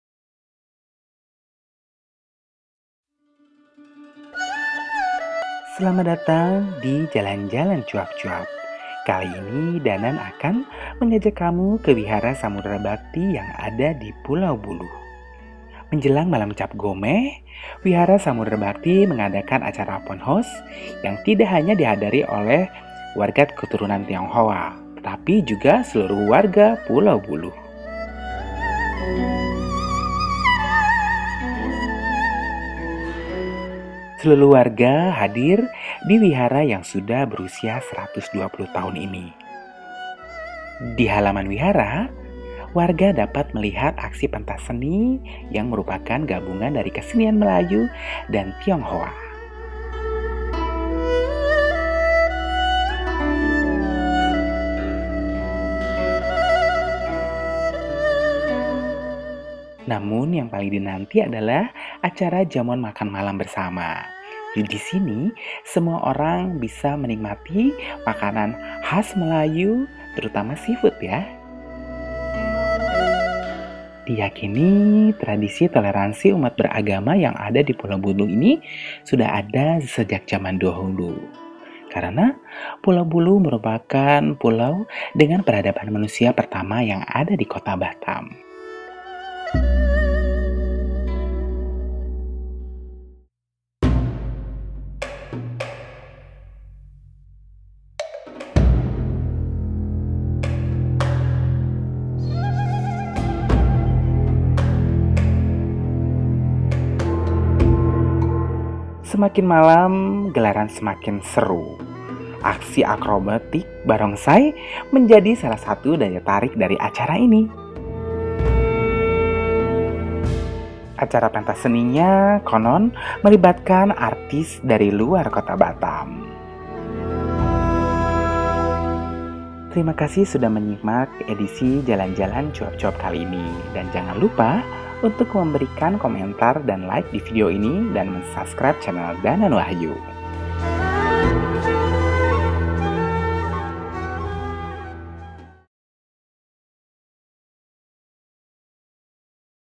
Jalan2Cuap2 - Cap Gomeh Vihara Samudra Bhakti Pulau Buluh
Toleransi beragama di Indonesia konon sudah ada sejak jaman dahulu. Mungkin salah satu buktinya ada di pulau Buluh kota Batam. Terbukti di pulau yang disebut sebagai pulau peradaban di kota Batam ini terdapat tradisi Cap Gomeh yang tak hanya melibatkan warga keturunan Tionghoa tapi seluru warga pulau yang beragama muslim dan suku Melayu.

Menjelang malam perayaan Cap Gomeh di Vihara Samudra Bakti yang berumur seratus tahun lebih diadakan jamuan makan malam dan pentas seni yang dihadiri oleh seluruh warga pulau.

Tak hanya perayaan dari persiapan juga melibatkan seluruh warga pulau. Menyaksikan semua aktivitas ini rasanya inilah toleransi umat beragama yang harus dijaga dan menjadi bagian dari budaya di negeri ini.

filmed by @dananwahyu